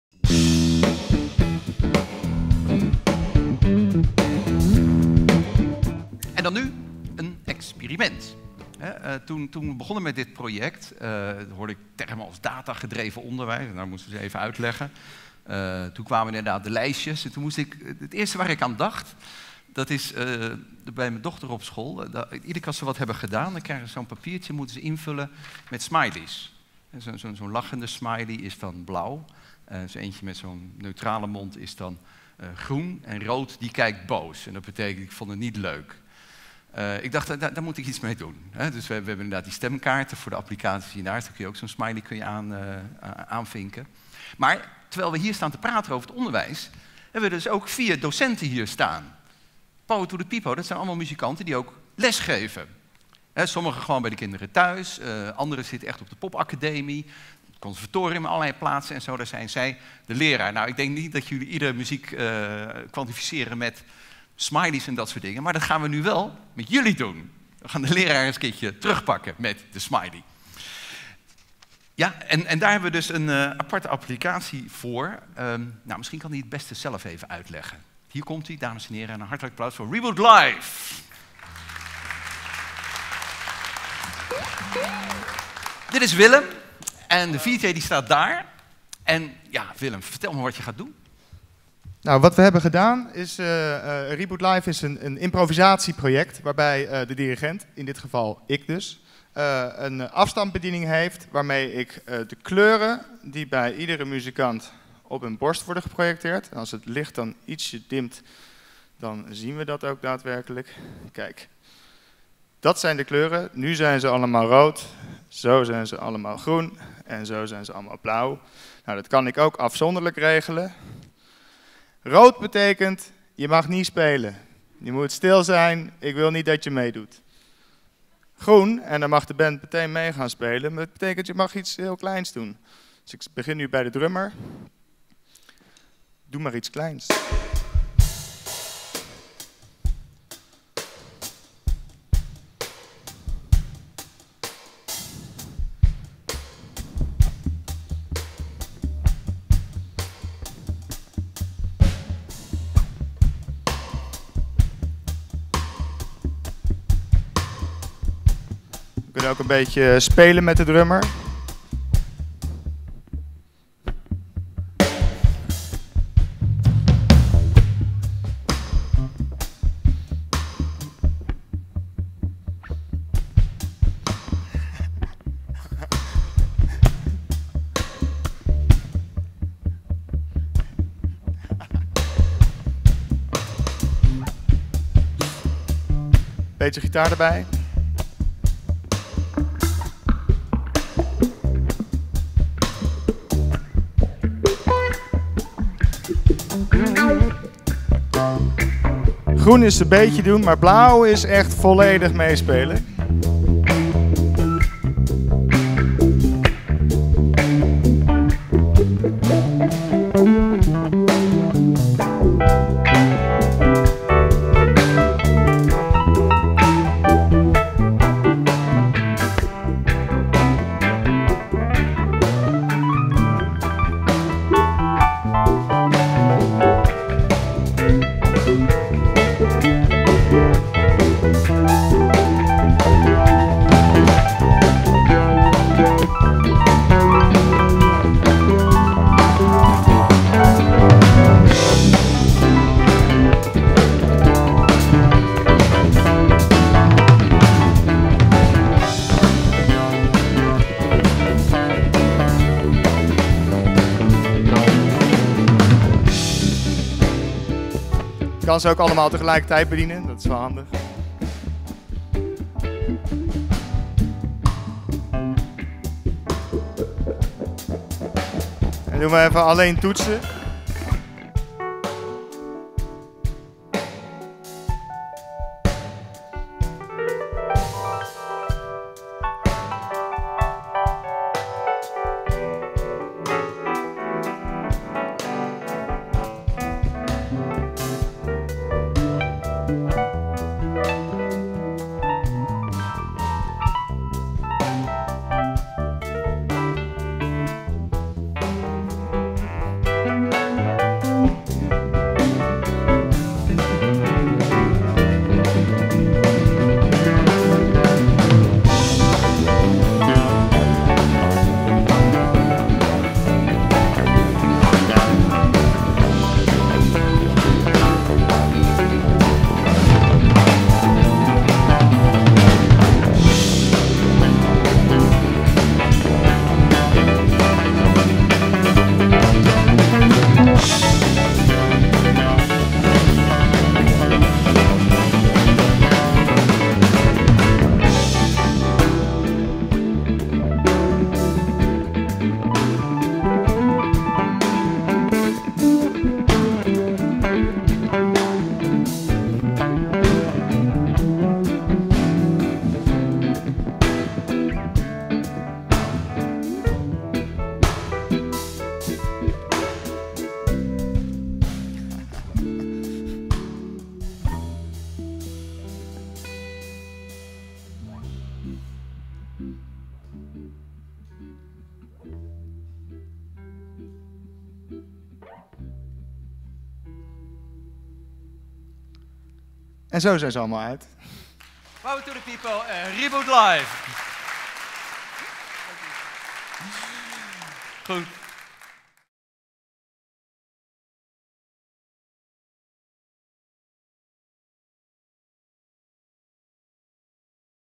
Tek Tok late night 5 7 datagedreven muziek met Reboot live
Tijdens deze Tek Tok late night over data in onderwijs deden we een experiment met een "data-gedreven band": Reboot live. Toen we begonnen aan dit thema met Kennisnet, was het eerste waar ik aan dacht die blauwe, groene en rode smileys die leerlingen kunnen aanvinken bij opdrachten: blij, neutraal, boos. Daar moesten we iets mee doen, want de leden van onze huisband Power to the Pipo zijn allemaal muziekleraar.

Tijdens de Talkshow moesten ze als leerling gehoorzamen aan de applicatie van Reboot Live. Hiermee wordt elke muzikant aangelicht: rood betekent "niet spelen", groen "doe rustig mee" en blauw "ga maar helemaal los!". Geen gemakkelijke opgave voor de muzikanten...